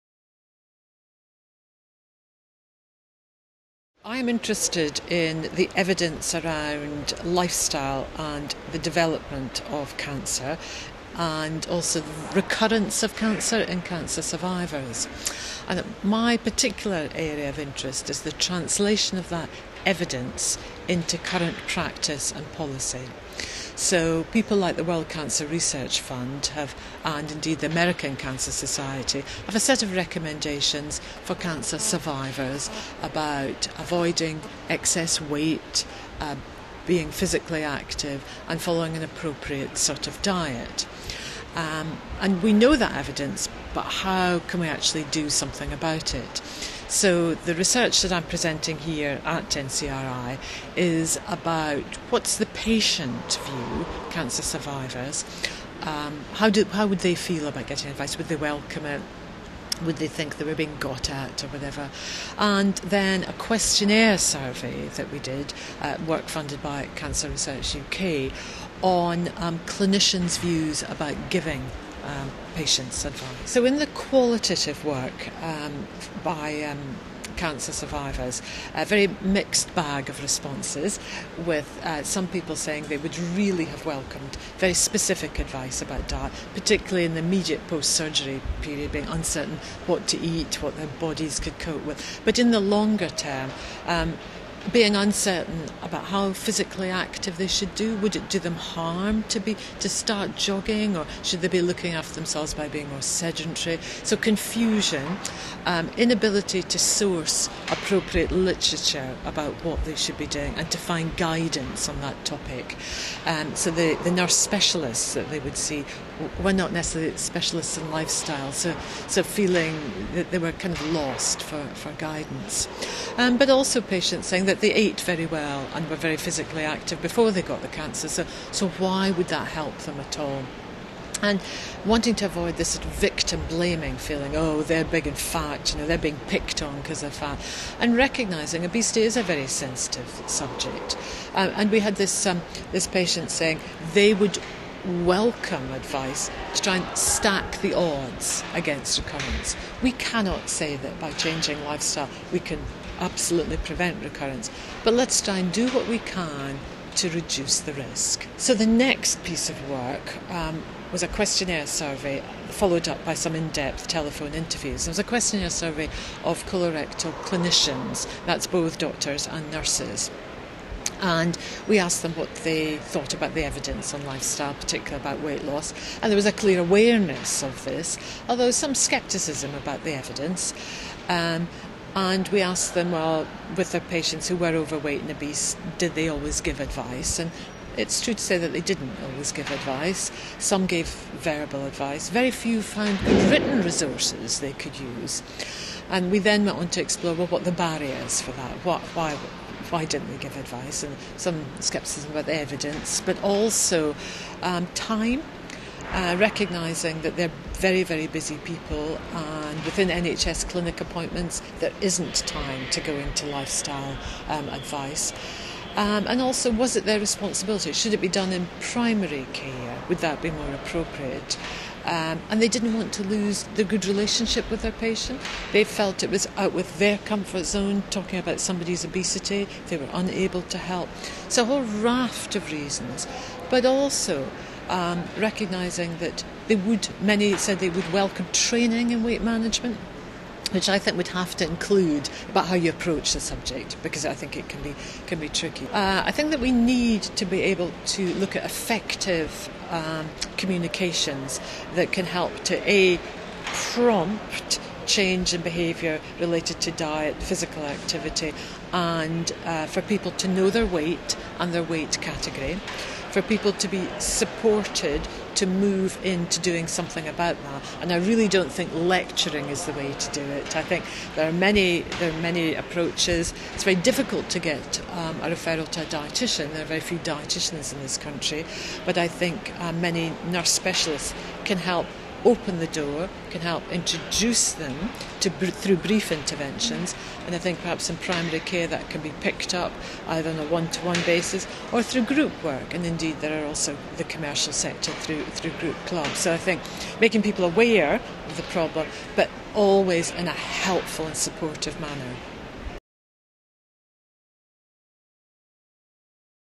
Professor Annie Anderson discusses the link between obesity and cancer
People who work in health care are failing to give patients crucial information about the link between obesity and cancer according to new research presented today (Tuesday) at the National Cancer Research Institute Cancer Conference (NCRI) in Liverpool.